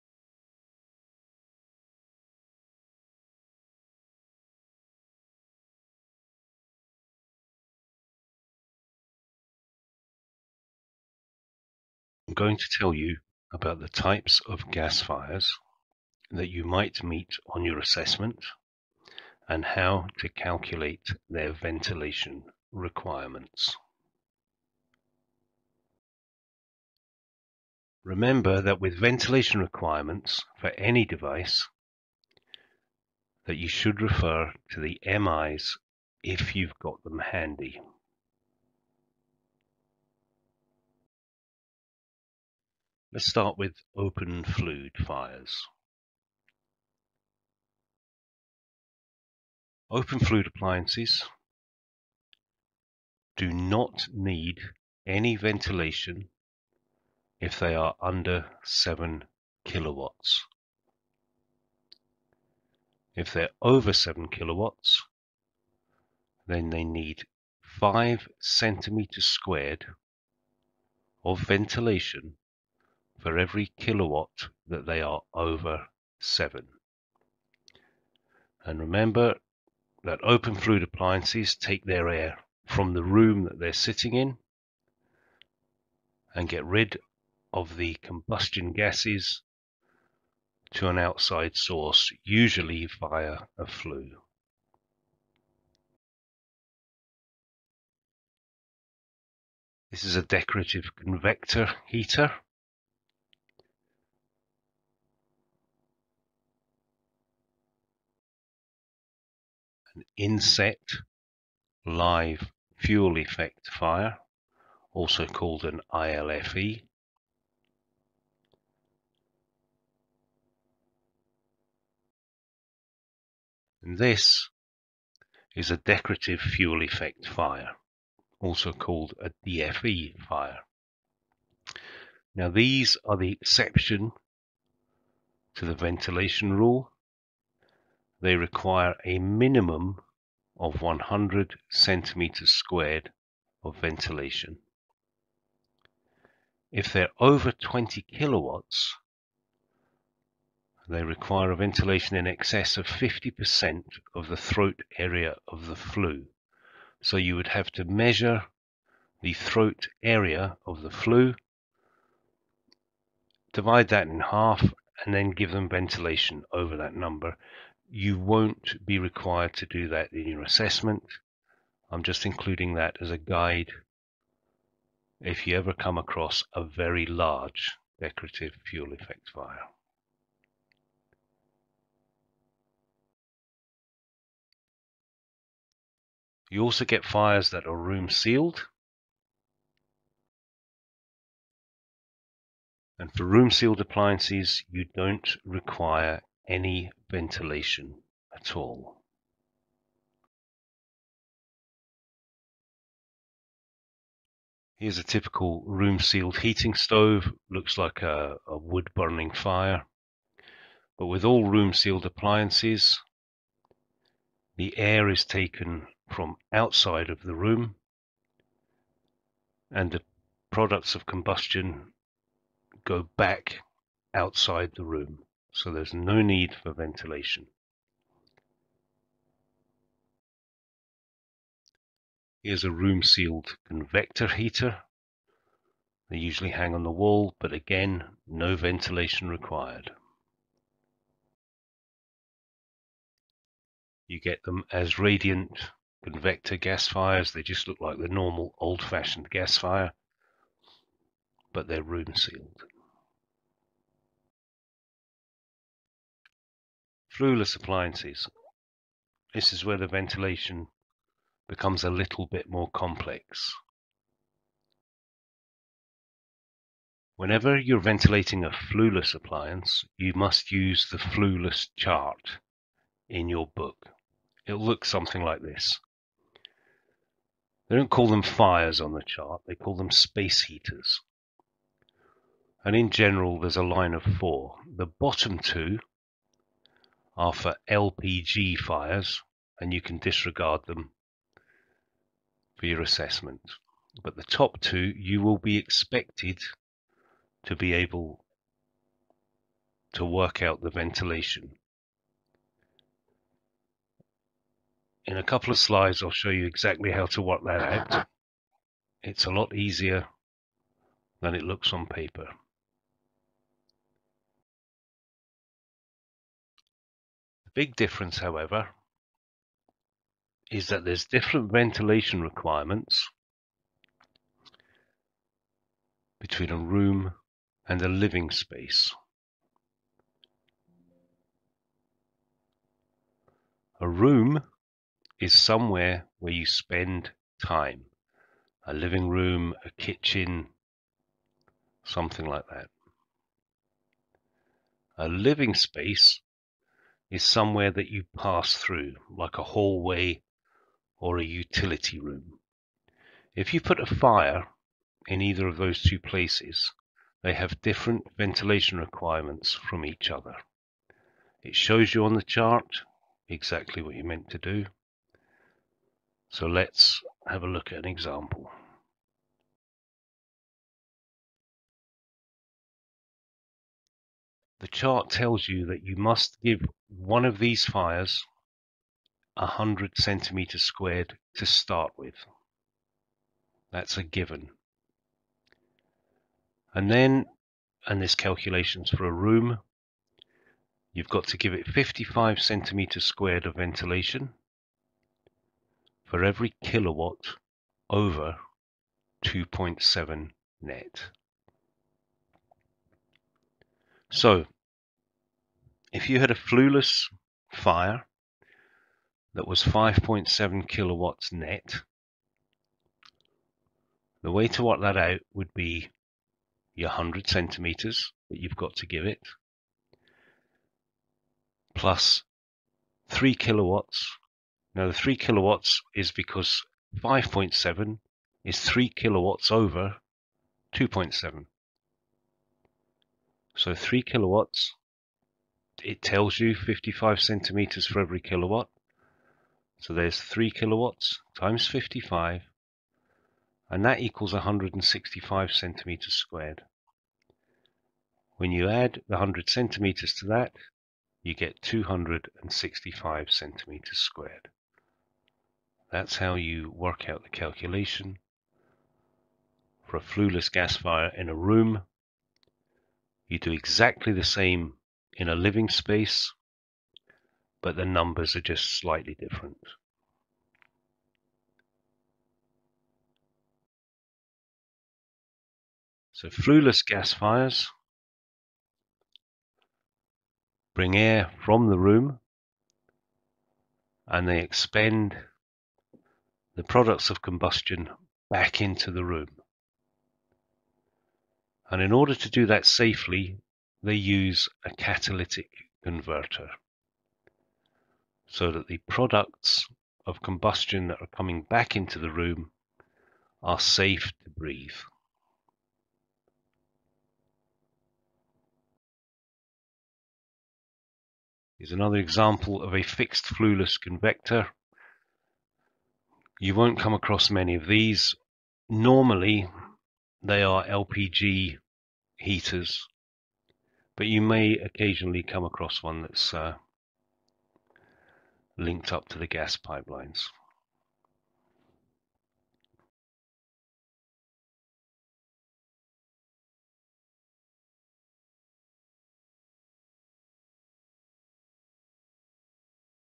ACS Fire Types and their ventilation
These are the types of gas fire you may encounter on your assessment. There is a description of each type and an explanation telling you how to calculate the ventilation needed.